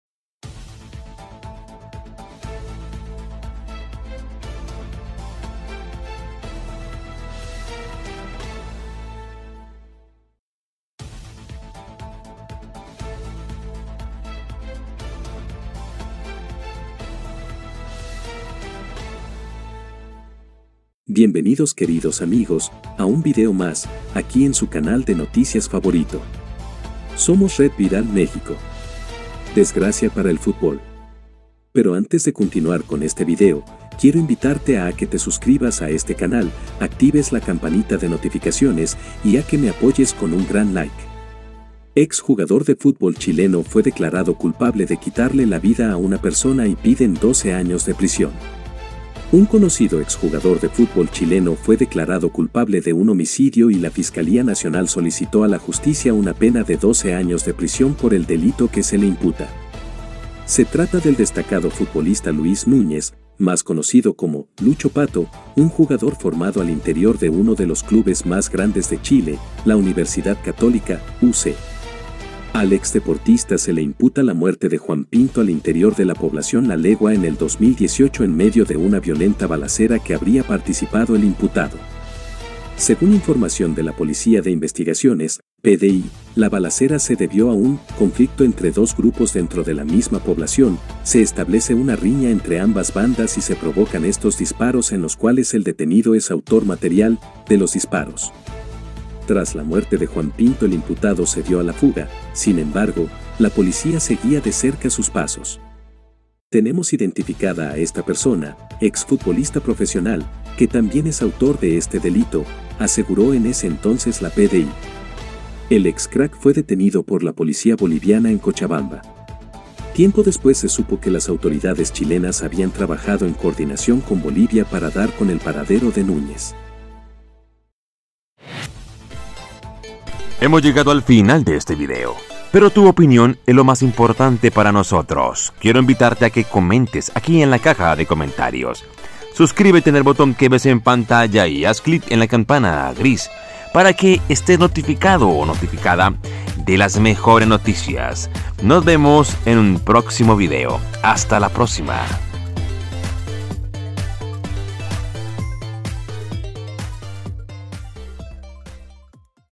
➕🖤¡ATENCIÓN! DESGRACIA PARA EL FÚTBOL CHILENO LA POLICIA ARRESTA AL MAXIMO GOLEADOR EN PLENO PARTIDO
🚨¡Atención! ACABAN DE CONFIRMAR  EL ARRESTO UNA DE LAS MÁXIMOS GOLEADORES DEL FÚTBOL.
!! BIENVENIDOS A @Somos Red Viral Mexico

Image

Ⓒ Video

Ⓒ Song ~

O interprete